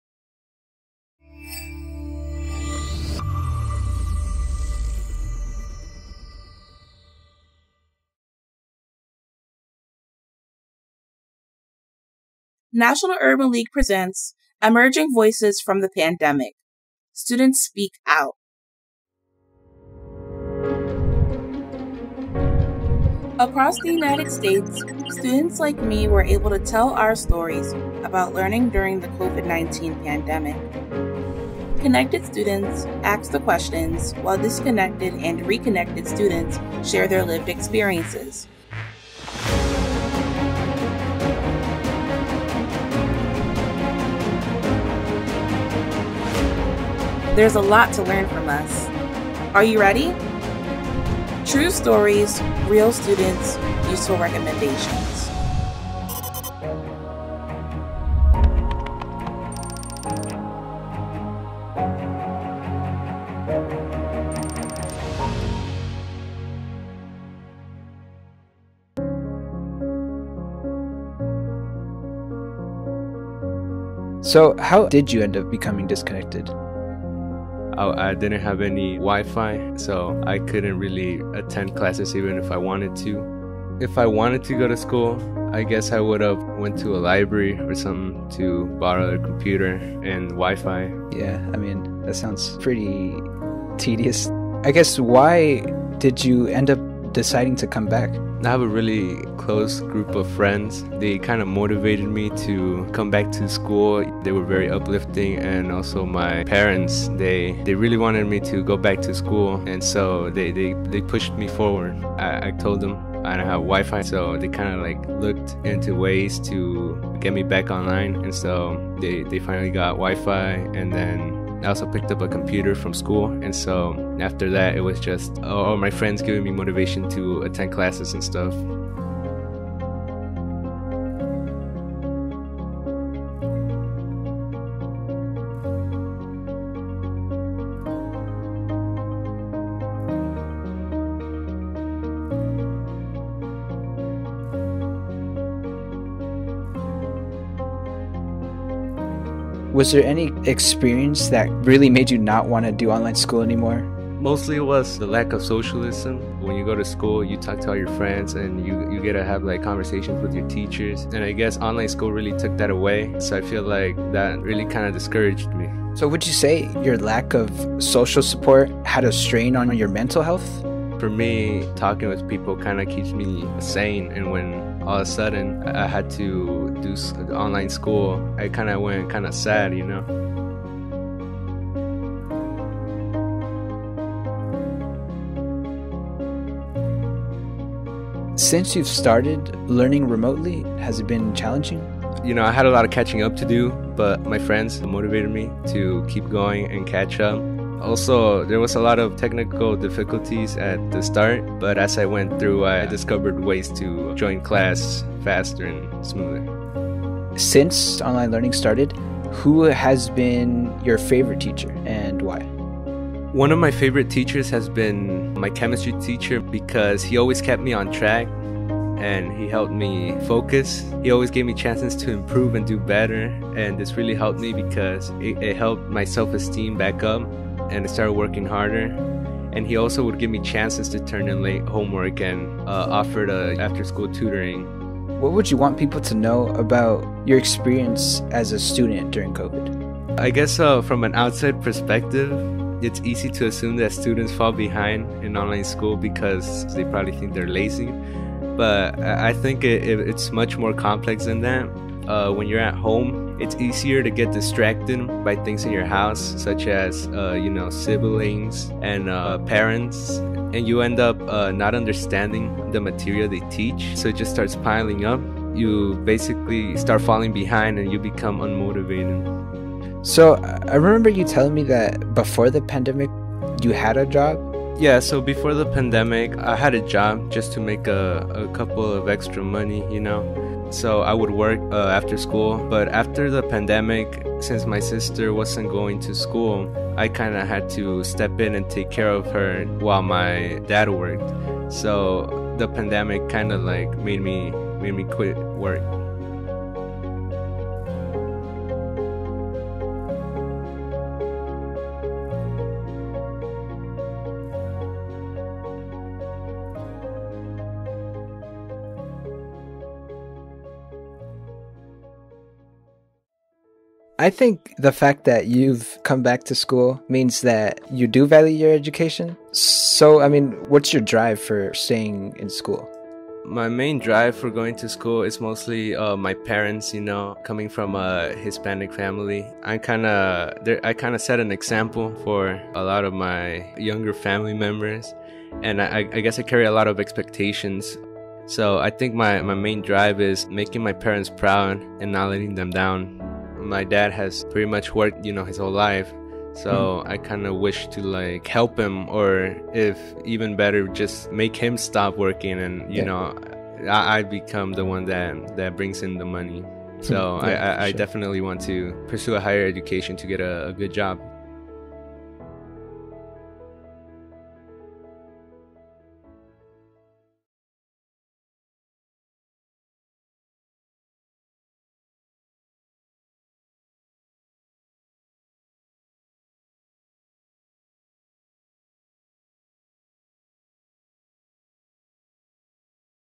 Unmasked Stories of Inequity | Emerging Voices From the Pandemic - Joel & Angel
This Week's Story: Joel & Angel
Two California students talk about the lack of technology at home, the importance of community, and the expectations of a Hispanic family.